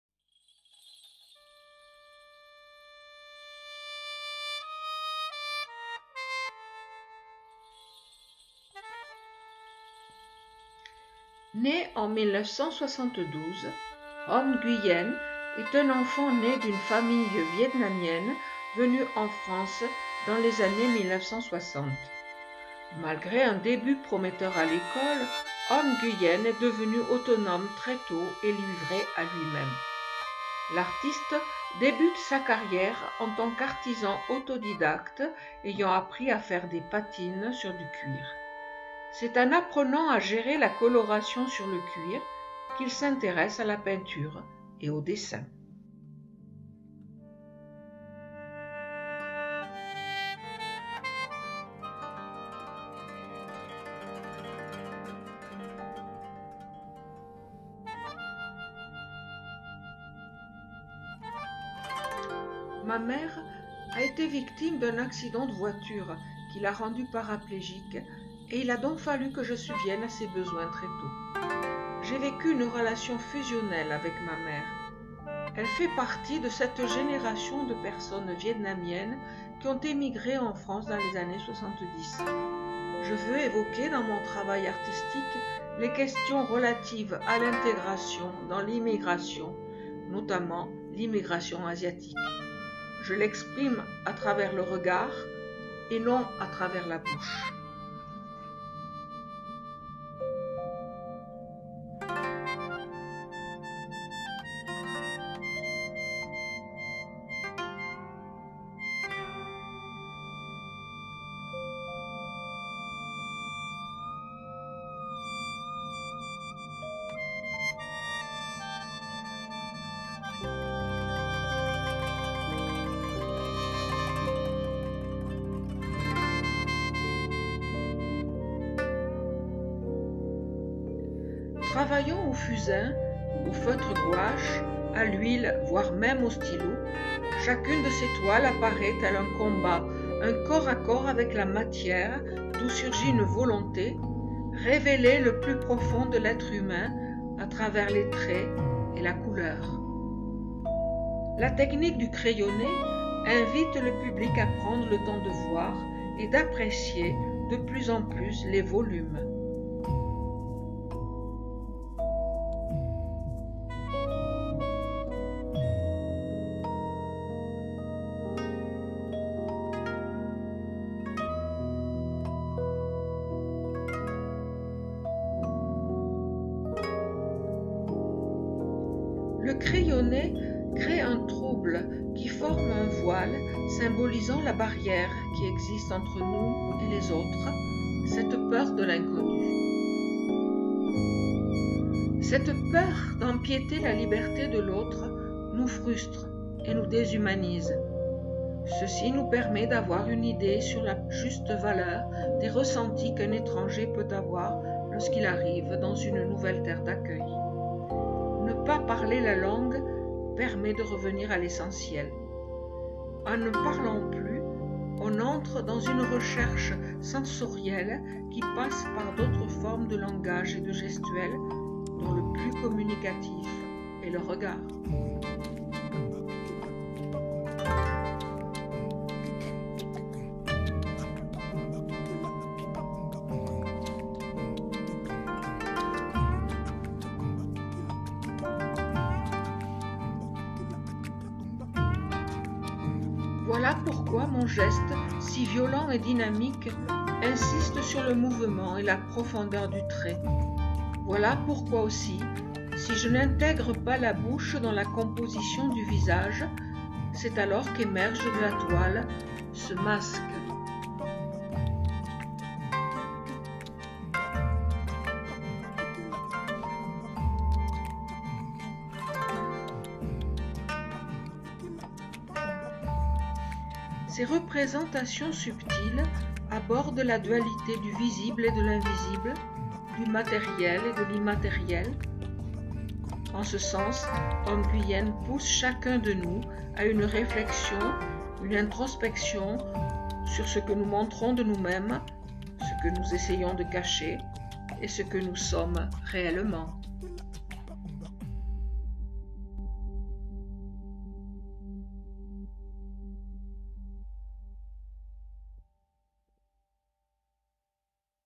Hom Nguyen par Anne-Marie
Première visite à l'expo : un agacement devant une technique que je trouve trop agressive. . . Recherche, lectures.  .  m'ont permis  de donner sens à ce travail  et respect à l'artiste .